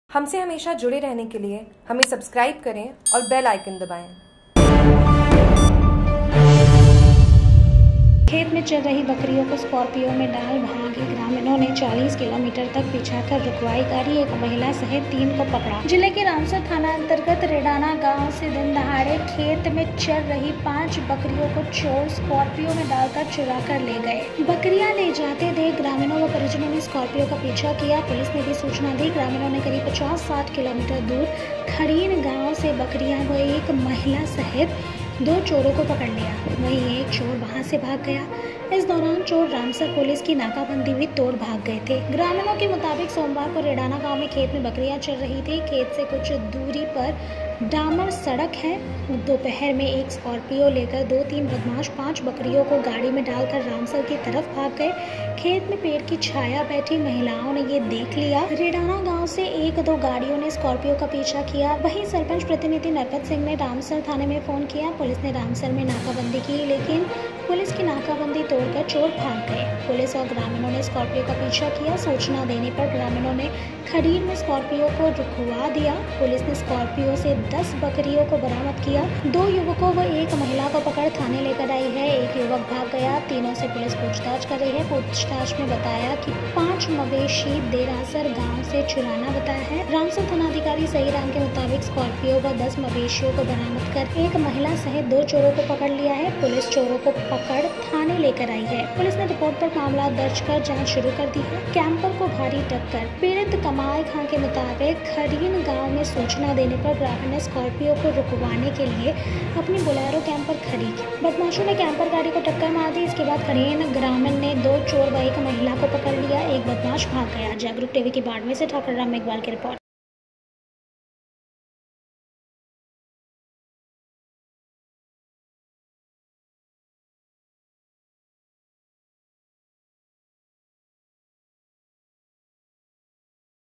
दिन दहाड़े खेत में चर रही 5 बकरियों को चोर स्कॉपियों में डाल कर चुरा कर ले गए पुलिस ने किया गिरफ्तार
जिले के रामसर थानान्तर्गत रेडाणा गांव से दिन दहाड़े खेत में चर रही 5 बकरियों को चोर स्कॉपियों में डाल कर चुरा कर ले गए। बकरियां ले जाते देख ग्रामीणों व परिजनों ने स्कार्पियों का पीछा किया। पुलिस को भी सूचना दी। ग्रामीणों ने करीब 50-60 किलोमीटर दूर खड़ीन गांव से बकरियां व एक महिला सहित 2 चोरों को पकड़ लिया। वहीं, एक चोर वहां से भाग गया। इस दौरान चोर रामसर पुलिस की नाकाबंदी भी तोड़ भाग गए थे।
ग्रामीण के मुताबिक सोमवार को रेडाणा गांव में खेत में बकरियां चर रही थीं। खेत से कुछ दूरी पर डामर सड़क है। दोपहर में एक स्कार्पियो लेकर दो-तीन बदमाश 5 बकरियों को गाड़ी में डालकर रामसर की तरफ भाग गए। खेत में पेड़ की छाया बैठी महिलाओं ने यह देख लिया। महिलाओं ने घरवालों को फोन किया। रेडाणा गांव से एक-दो गाड़ियों ने स्कार्पियो का पीछा किया। वहीं, सरपंच प्रतिनिधि नरपत सिंह ने रामसर थाने में फोन किया। पुलिस ने रामसर में नाकाबंदी की लेकिन, पुलिस की नाकाबंदी तोड़कर चोर भाग गए।
पुलिस और ग्रामीणों ने स्कार्पियो का पीछा किया। सूचना देने पर ग्रामीणों ने खड़ीन में स्कार्पियो को रुकवा दिया। पुलिस ने स्कार्पियो से 10 बकरियों को बरामद किया। दो युवकों व एक महिला को पकड़ थाने लेकर आई है। एक युवक भाग गया। तीनों से पुलिस पूछताछ कर रही है। पूछताछ में बताया कि 5 मवेशी देरासर गांव से चुराना बताया है।
रामसर थानाधिकारी सहीराम के मुताबिक स्कार्पियो व 10 मवेशियों को बरामद कर एक महिला सहित दो चोरों को पकड़ लिया है। पुलिस चोरों को पकड़ थाने लेकर आई है। पुलिस ने रिपोर्ट पर मामला दर्ज कर जांच शुरू कर दी है।